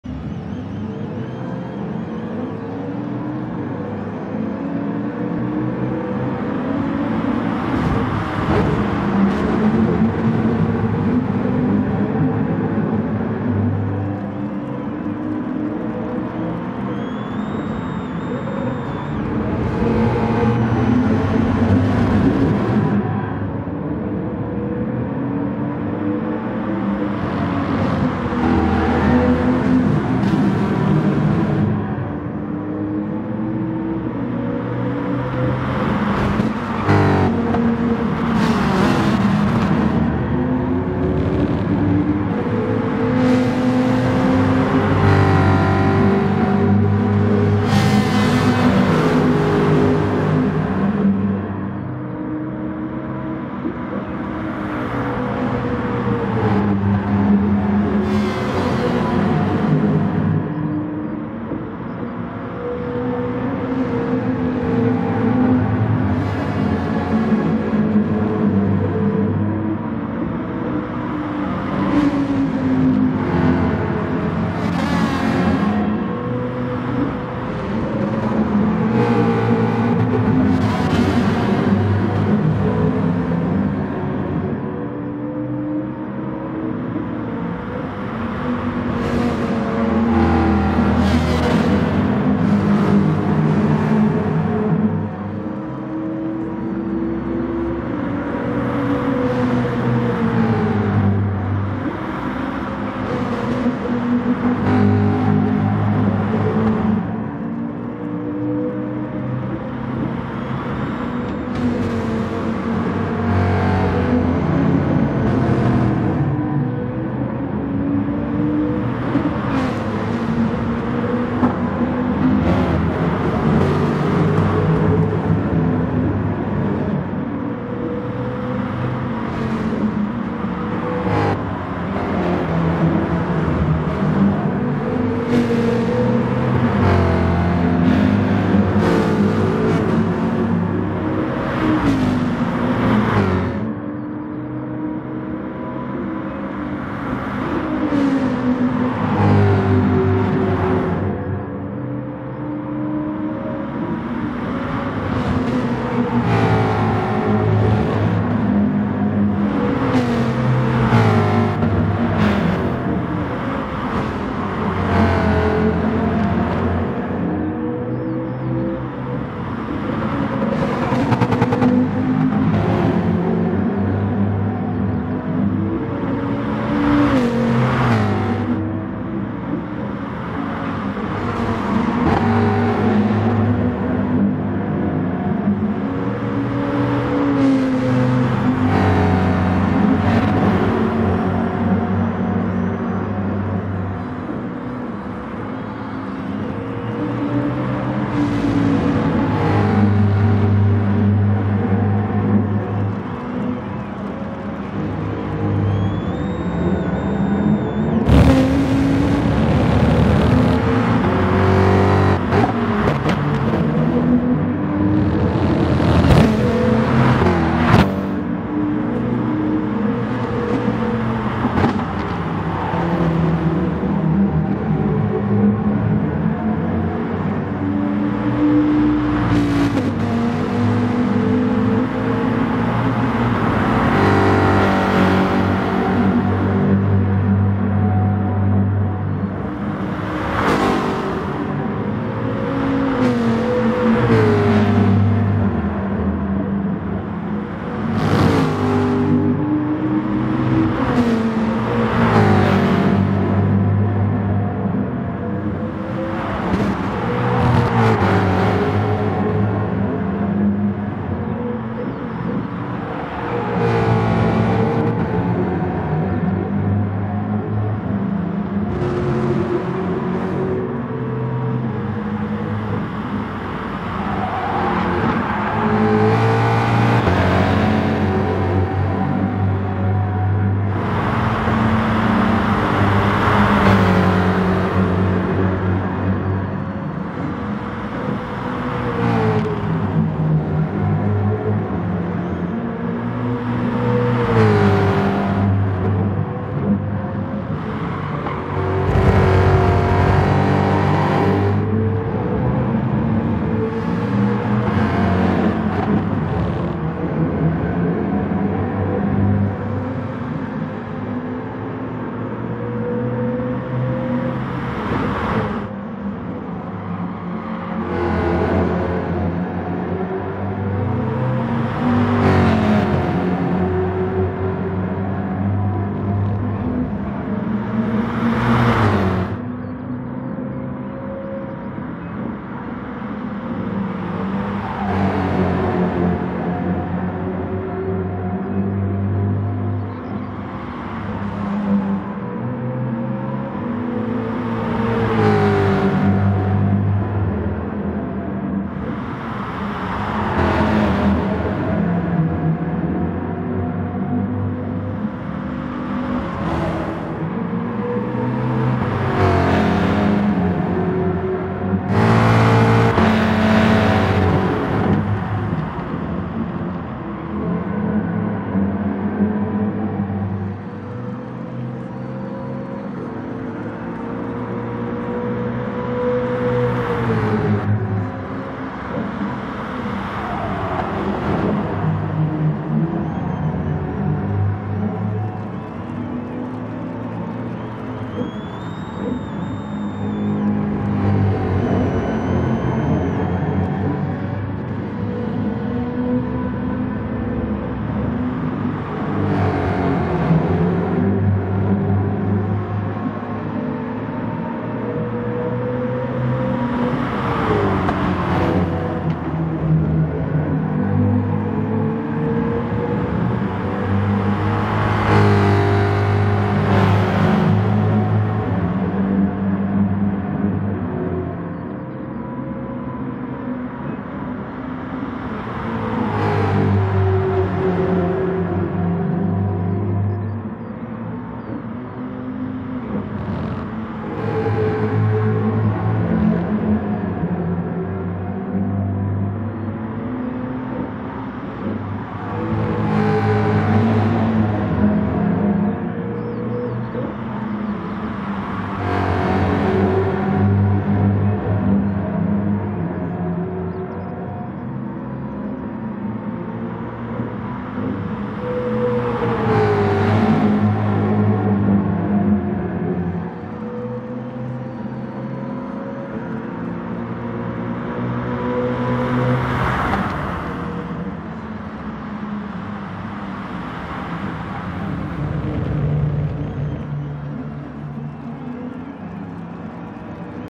ResetEra friendly sprint race @ Brand Hatch
Great nose to tail action from start to finish. Great fun race